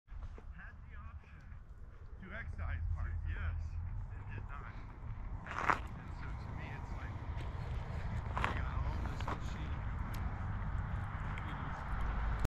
Harper Lake Feb 16 2024
Early morning, no real sunrise due to extremely heavy fog.
Two runners are out, one in shorts, even though it's 26 degrees
at 6:30 AM.

It's Colorado.